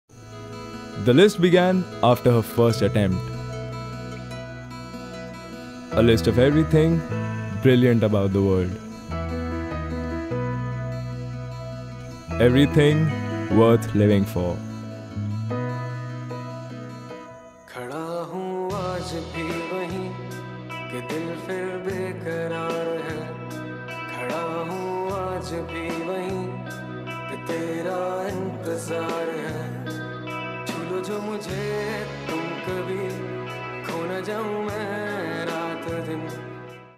Every Brilliant Thing | Immersive Theatre Teaser | The Drama Club, BITS Goa
Trailer for an Adapted Immersive Theatre Production, based on Duncan Macmillan's play, performed on 26th August, 2019